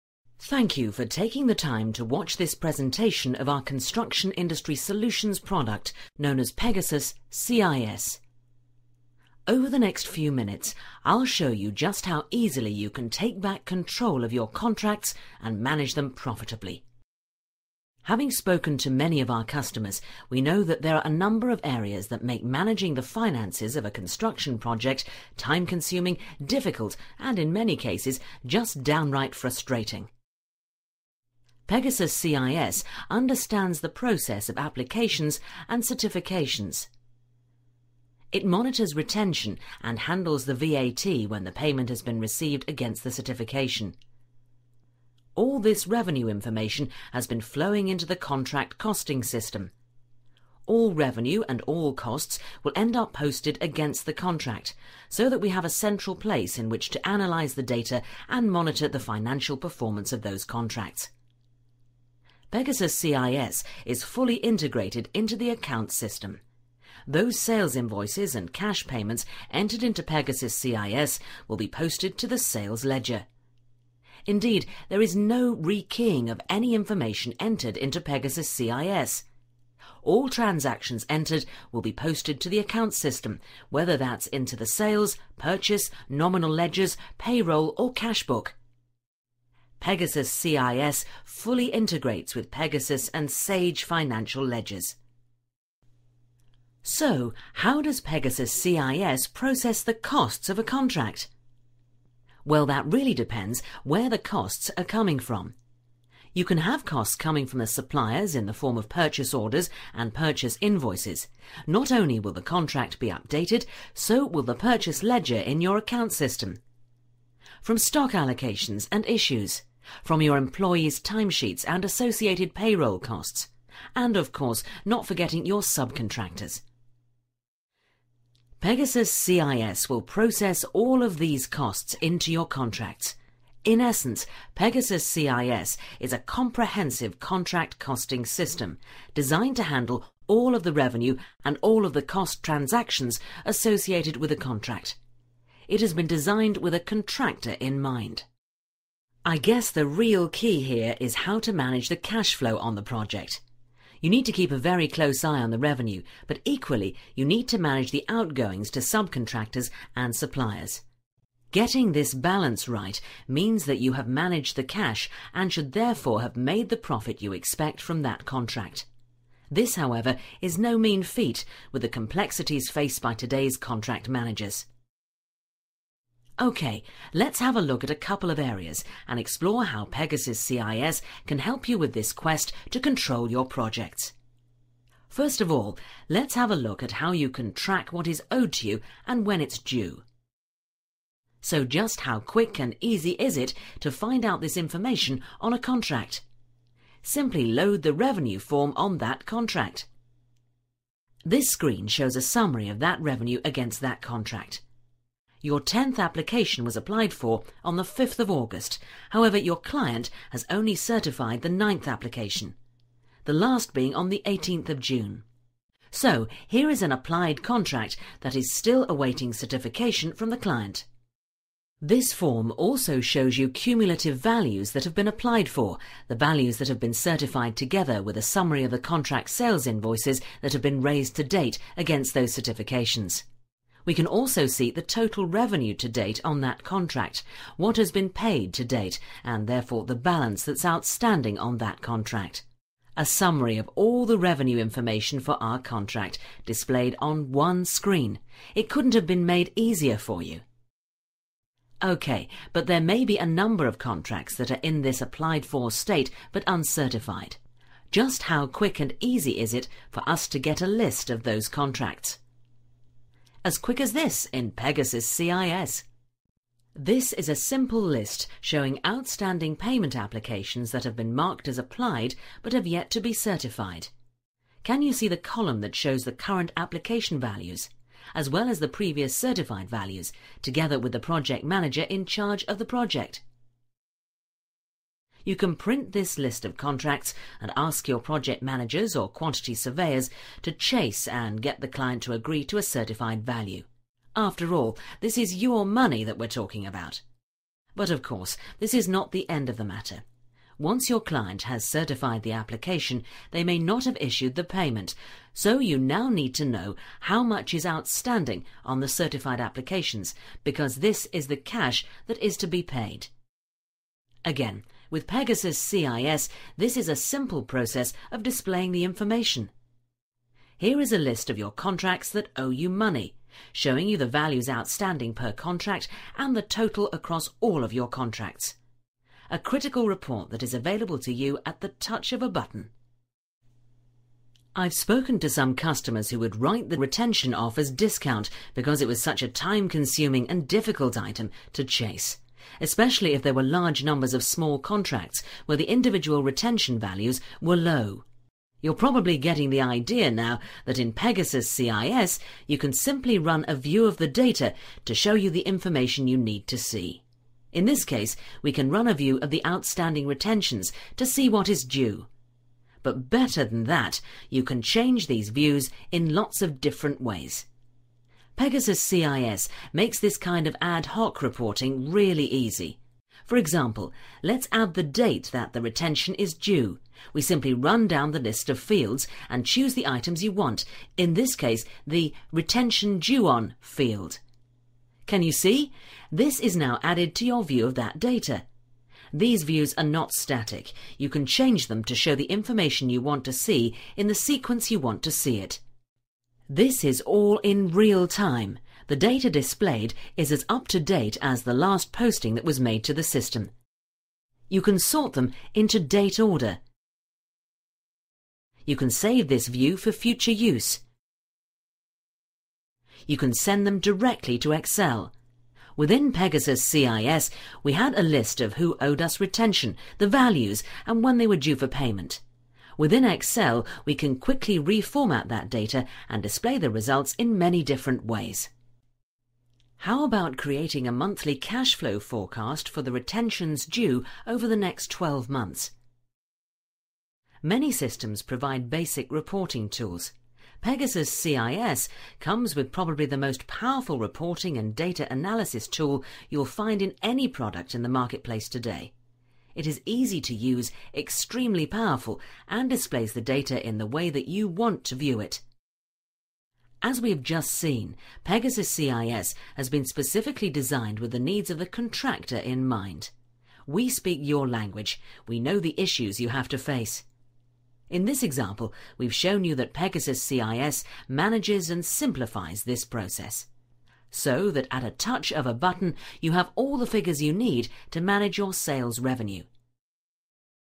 Pegasus Software - CIS | Business Software
Pegasus construction software supports major organisations.  It lets you view the profitability of the contract and calculated costs to completion.  This business software is best suited for construction and contracting industries.  It forms an end to end solution for reducing the costs of construction and maintenance.  This is a fully integrated application that provides critical accounting, operations and project management data.  It involves superior technology to offer stability and flexible user interface increases efficiency.  It gives you instant access to up to date information that allows you to manage all your contracts.  Management information system feature of this business software makes it easy to analyse key information such as costs or revenues, by contract, etc.  It integrates your financial information into Microsoft excel so you can avoid re-keying.